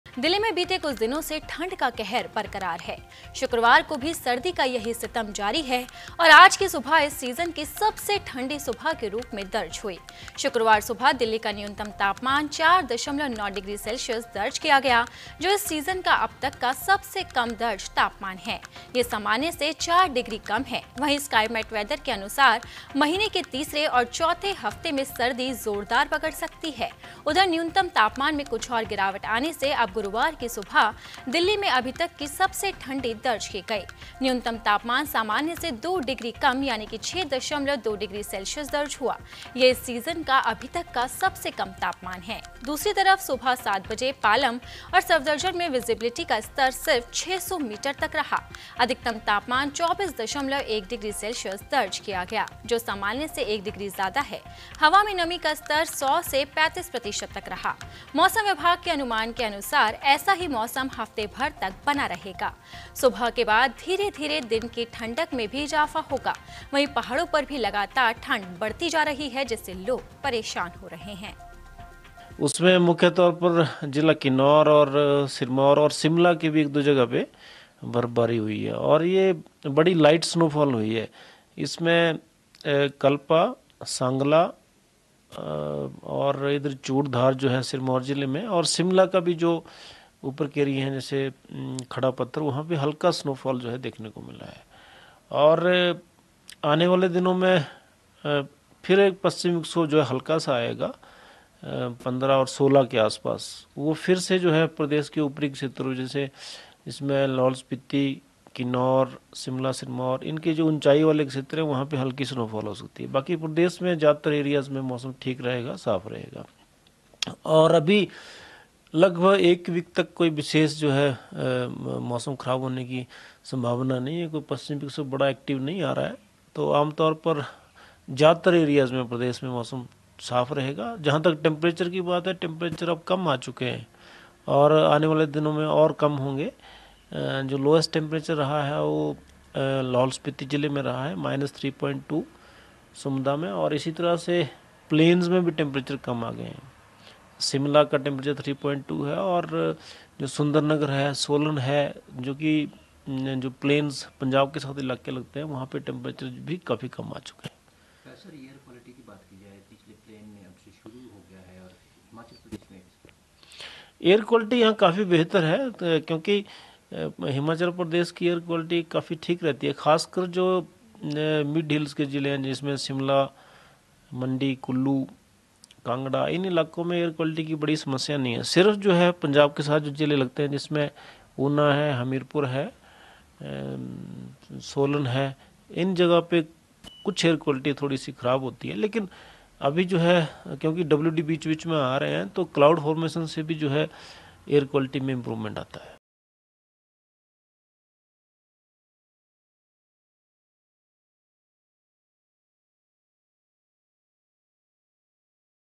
Weather Update: दिल्ली में लुढ़का पारा, इस दिन से पड़ेगा टॉर्चर। IMD। Snow
Weather Update: दिल्ली में बीते कुछ दिनों से ठंड का कहर बरकरार है...शुक्रवार को भी सर्दी का यही सितम जारी है और आज की सुबह इस सीजन की सबसे ठंडी सुबह के रूप में दर्ज हुई है...शुक्रवार सुबह दिल्ली का न्यूनतम तापमान 4.9 डिग्री सेल्सियस दर्ज किया गया जो इस सीजन का अब तक का सबसे कम दर्ज तापमान है। यह सामान्य से 4 डिग्री कम है।स्काईमेट वेदर के अनुसार महीने के तीसरे और चौथे सप्ताह में सर्दी जोर पकड़ सकती है। उधर न्यूनतम तापमान में कुछ और गिरावट आने से अब गुरुवार की सुबह दिल्ली में अभी तक की सबसे ठंडी दर्ज की गई।न्यूनतम तापमान सामान्य से दो डिग्री कम 6.2 डिग्री सेल्सियस दर्ज हुआ। यह इस सीजन का अभी तक का सबसे कम है। दूसरी तरफ सुबह सात बजे पालम और सफदरजंग में विजिबिलिटि का स्तर सिर्फ 600 मीटर तक रह गया।अधिकतम तापमान 24.1 डिग्री सेल्सियस रहा, जो सामान्य से एक डिग्री ज्यादा है। हवा में नमी का स्तर 100 से 35 प्रतिशत तक रहा...मौसम विभाग का अनुमान है कि ऐसा ही मौसम सप्ताह भर बना रहेगा। सुबह के बाद धीरे धीरे दिन की ठंडक में भी इजाफा होगा।...वहीं पहाड़ों पर भी लगातार ठंड बढ़ती जा रही है...